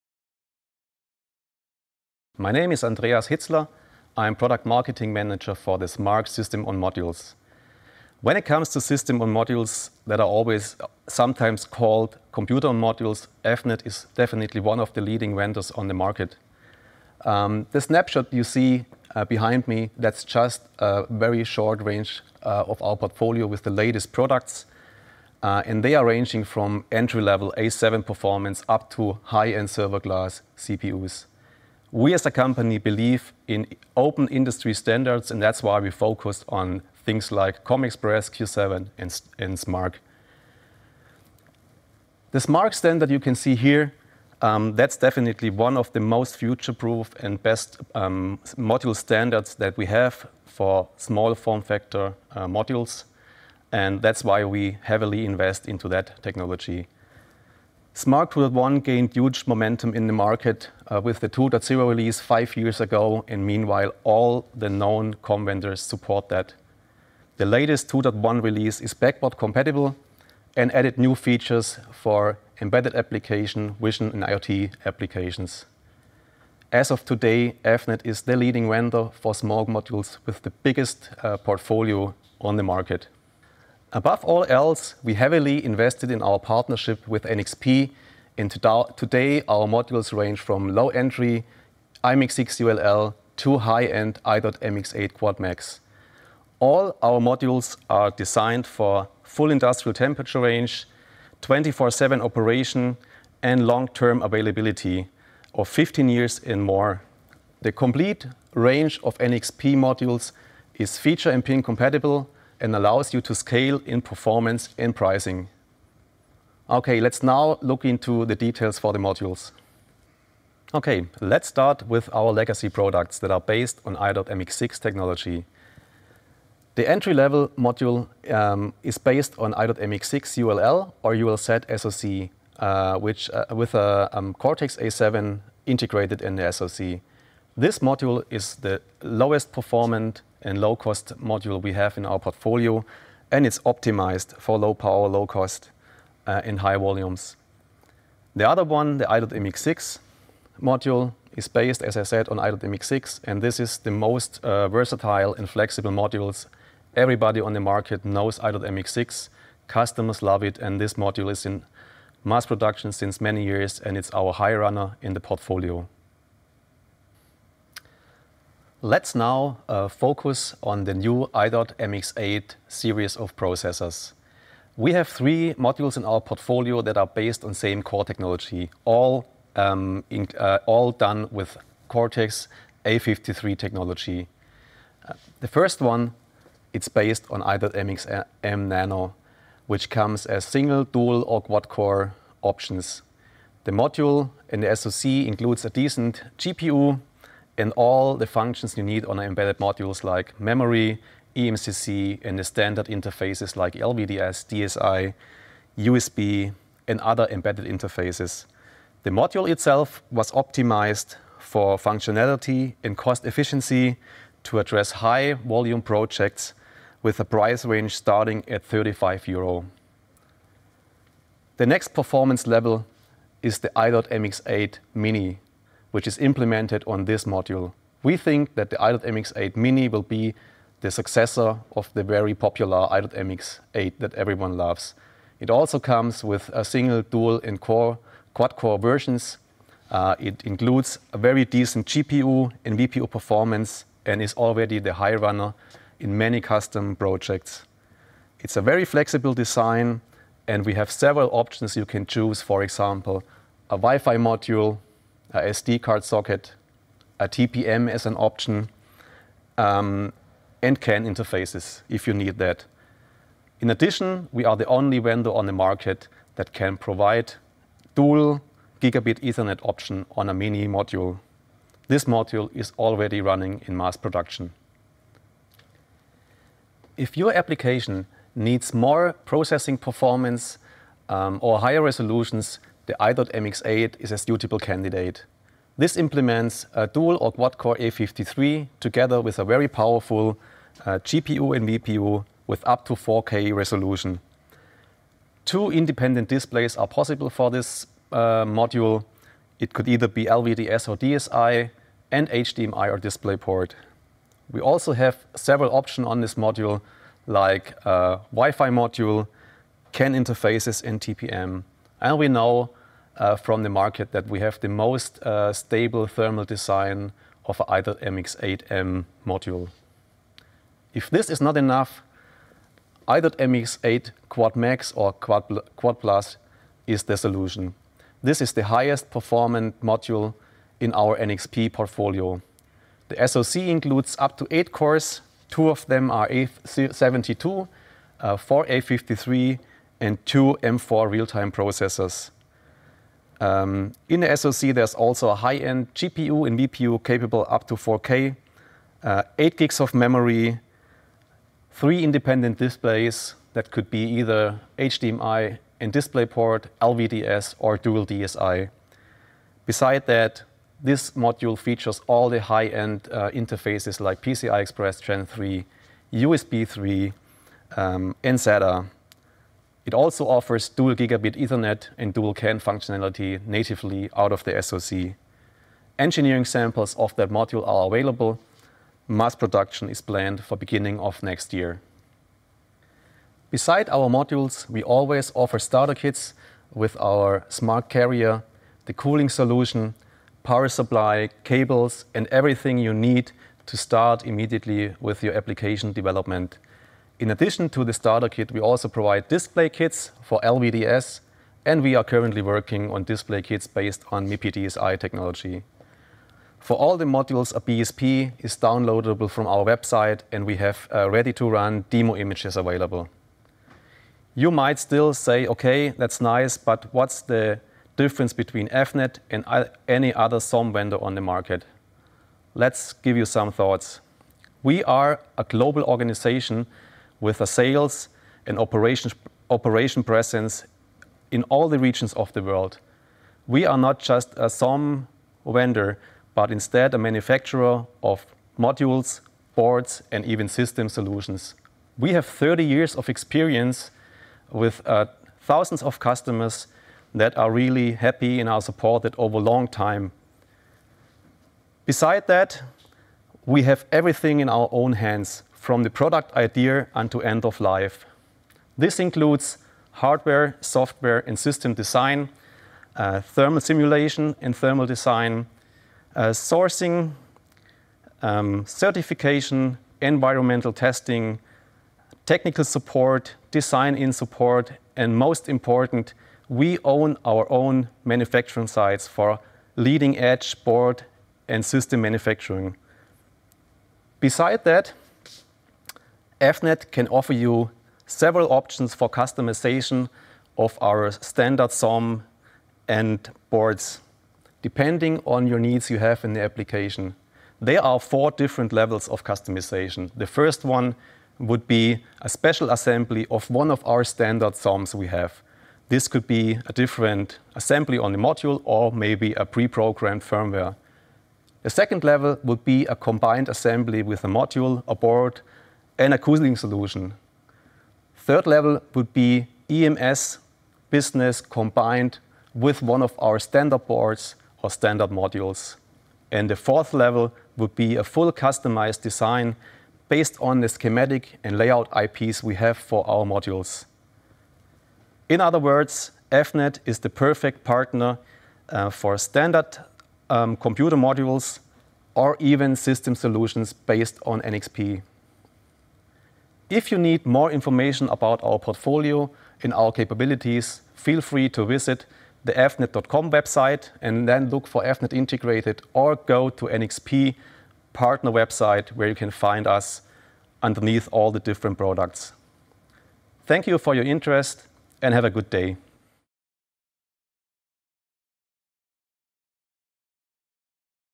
SMARC by Avnet Integrated | An overview of our modules
In this video Andreas Hitzler, Product Marketing Manager for SMARC at Avnet Integrated, gives an overview of the current range of SMARC form factor modules available to customers.  Recorded at our Global Technology Campus in Stutensee, Germany, where we design and manufacture our embedded modules for customers worldwide.